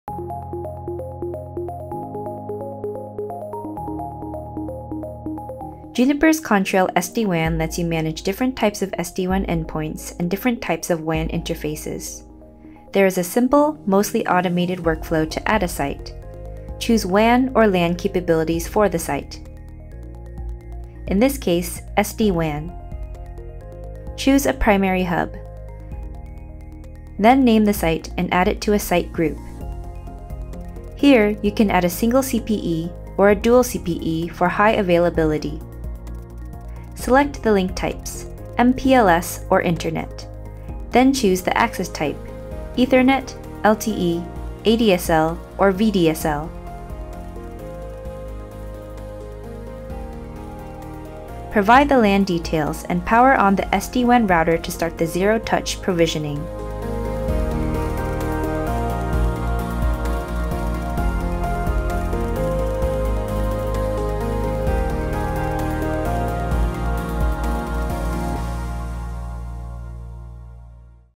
15 Features in 15 Minutes: How to Add WAN links to your Contrail SD-WAN Deployment
Contrail SD-WAN's workflow to add WAN links to a site is seamless and intuitive. You can get it done in a matter of minutes.
Watch the full 15 Features in 15 Minutes for SD-WAN playlist